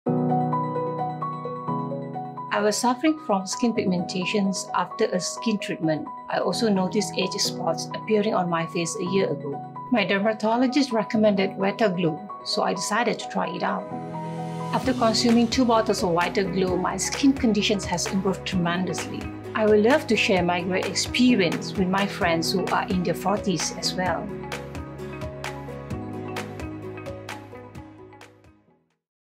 Age Gracefully and Glow-mazingly with Total Image Whita Glo - Glutathione Supplement
Choose the right beauty product to look glow-some at every age! Formulated with the highest dosage of OPITAC™ L-Glutathione (400mg) in the market, Whita Glo aids to reduce the appearance of dark spots and maintain a youthful, natural glow.